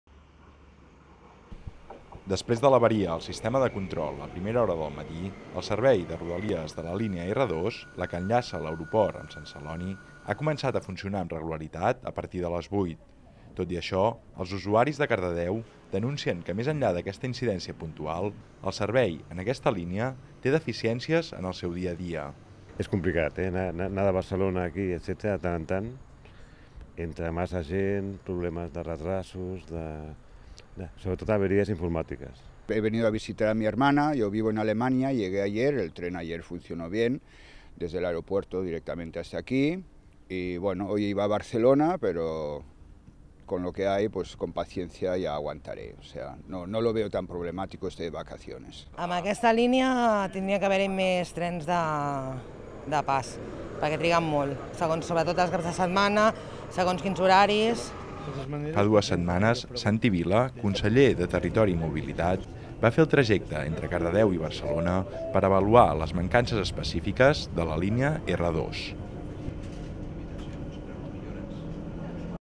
Els usuaris de la línia R2 de rodalies critiquen el servei al seu pas per Cardedeu
L'avaria del passat dijous al matí va deixar sense trens els usuaris de les diferents línies de rodalies. Aquesta incidència se suma a les queixes del servei de renfe a la línia R2 al seu pas per Cardedeu.